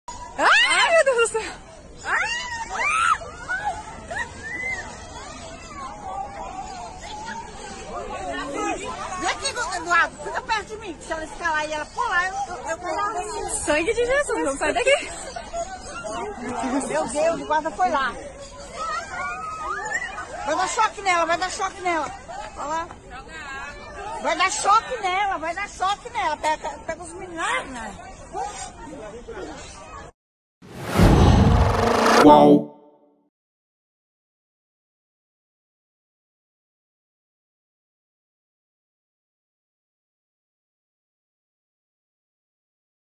Onça-pintada escala fosso e quase alcança visitantes no Zoológico de Brasília; veja vídeo
Uma onça-pintada escalou a pilastra do fosso onde fica e quase alcançou os visitantes do Zoológico de Brasília, nesta quinta-feira (28).